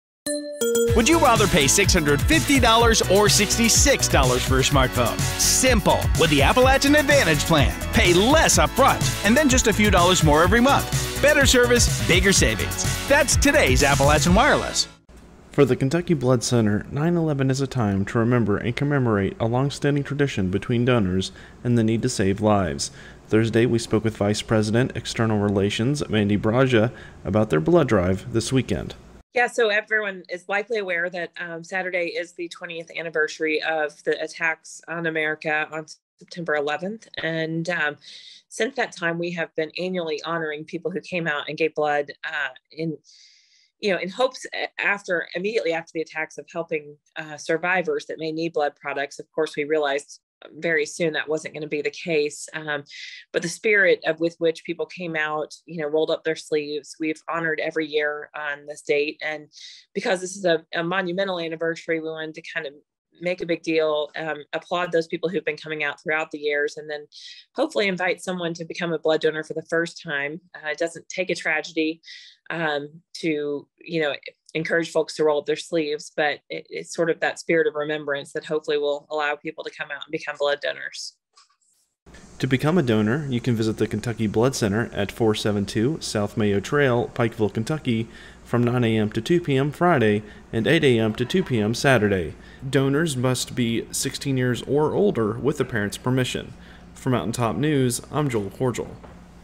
Kentucky Blood Centers Remember 9/11 with Annual Blood Drive
For the Kentucky Blood Center, 9/11 is time to remember and commemorate a long-standing tradition between donors and the need to save lives.